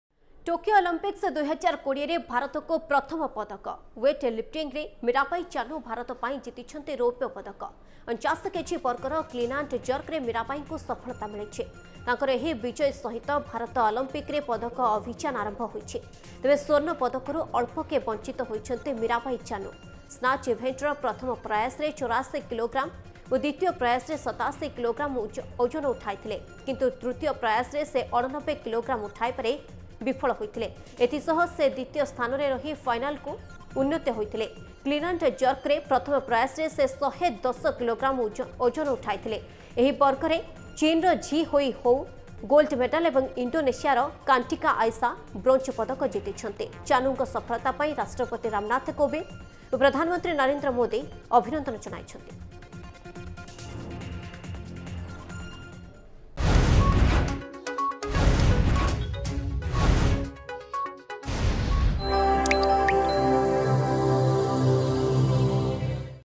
ଟୋକିଓ ଅଲିମ୍ପିକ୍ସ-୨୦୨୦ରେ ଭାରତକୁ ପ୍ରଥମ ପଦକ
ଟୋକିଓ ଅଲିମ୍ପିକ୍ସ-୨୦୨୦ରେ ଭାରତକୁ ପ୍ରଥମ ପଦକ । ୱେଟ ଲିଫ୍ଟିଂଗରେ ମୀରାବାଈ ଚାନୁ ଭାରତ ପାଇଁ ଜିତିଛନ୍ତି ରୌପ୍ୟ ପଦକ । ୪୯ କେଜି ବର୍ଗର କ୍ଲିନ୍‌ ଆଣ୍ଡ ଜର୍କରେ ମୀରାବାଈଙ୍କୁ ସଫଳତା ମିଳିଛି ।